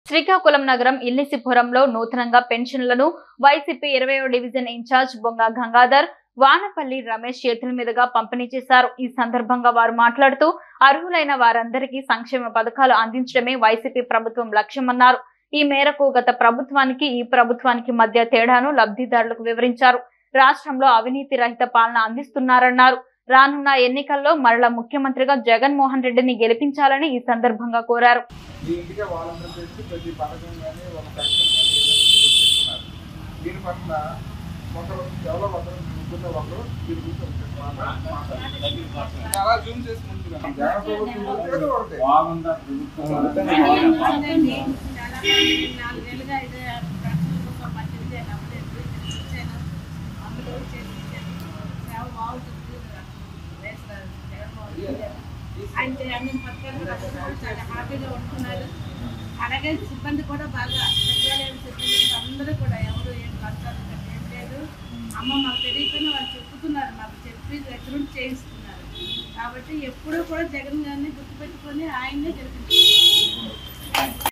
ఇల్లిసిపురంలో నూతన పెన్షన్ పంపిణీ చేస్తున్న 20వ డివిజన్ ఇన్చార్జి బొంగ గంగాధర్ ||New Tv Telugu
Watch on  AP fiber 109